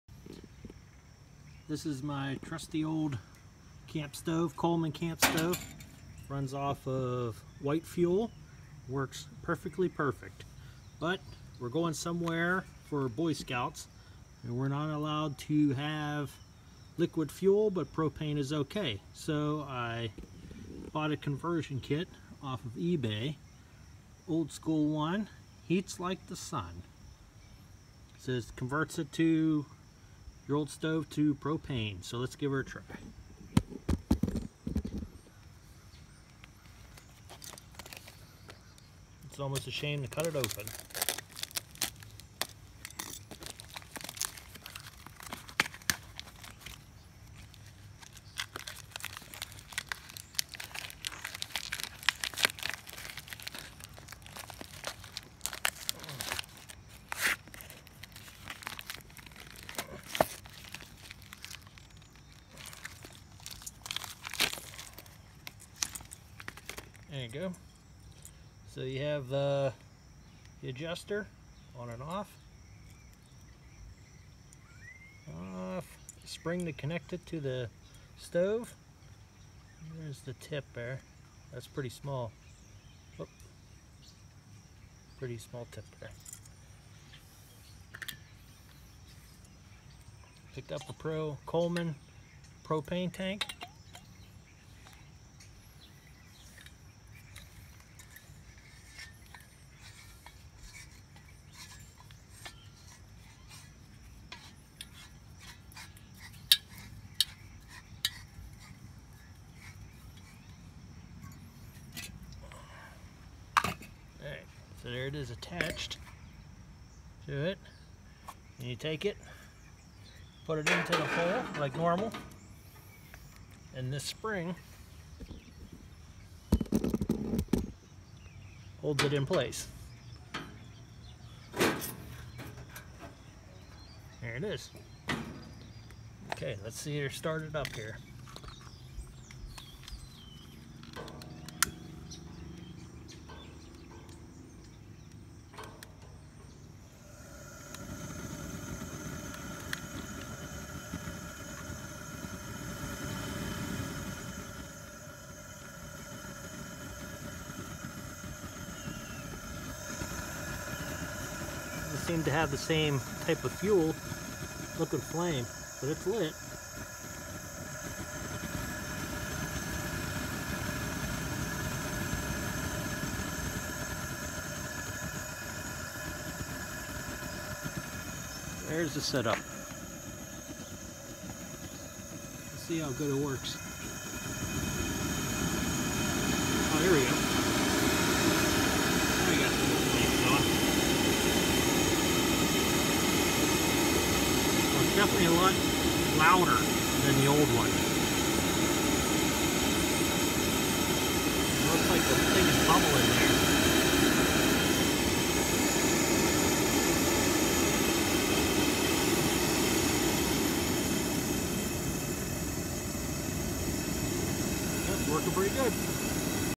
Coleman fuel to propane conversions
Convert my Coleman stove from white fuel to propane. Super hot!!